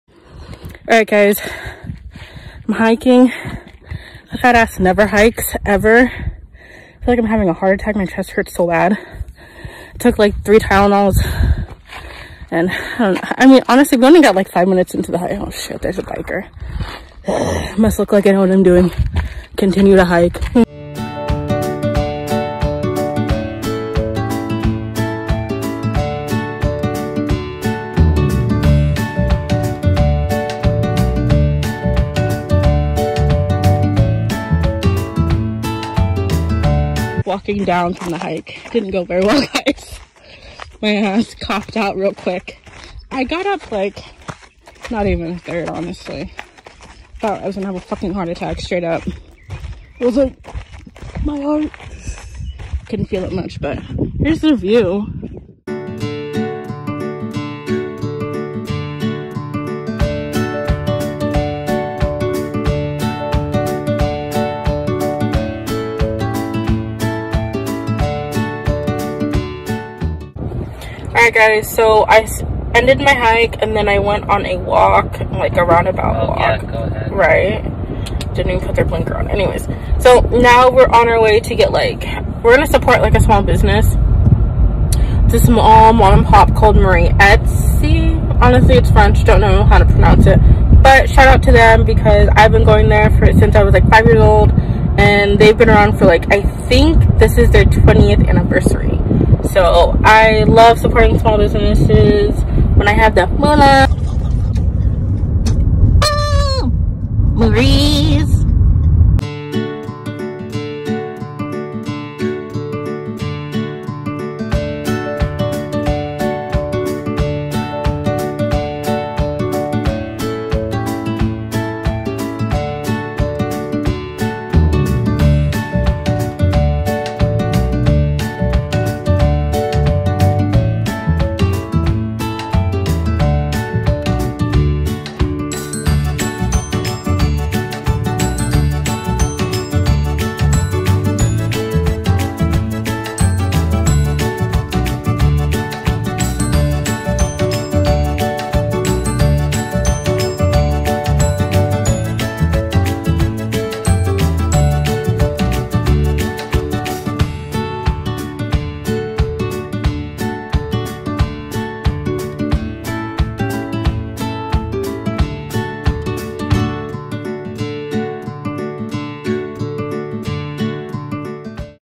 Just a Day in Quarantine
Hi guys so as I am in the house bored, I have been trying to work out little by little and support local businesses. The one I love to support and anyone in LA should really try is Marie Et Cie! This place is amazing and the desserts are to die for literally all of them are just delectable delicious goodness. Try them out if you guys get a chance they're coffee puts Starbucks to shame.

Hope you guys enjoy and are staying safe.